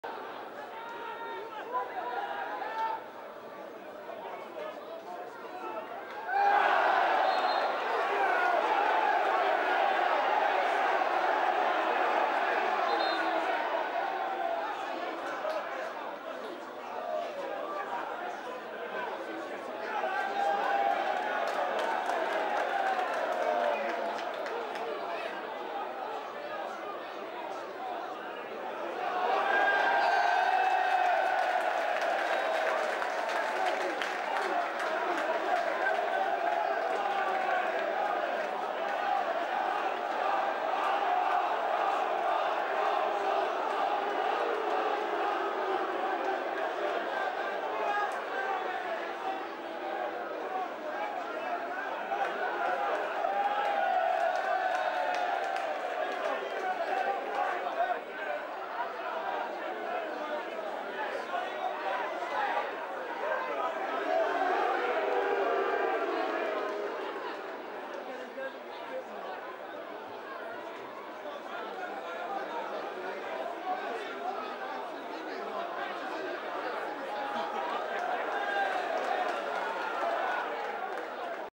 Referee injured at Sixfields
Referee Darren Sheldrake leaves the field injured during the Northampton Town v Morecambe game. This is the official You Tube channel of Northampton Town Football Club, playing in Sky Bet Football League 2 at Sixfields Stadium.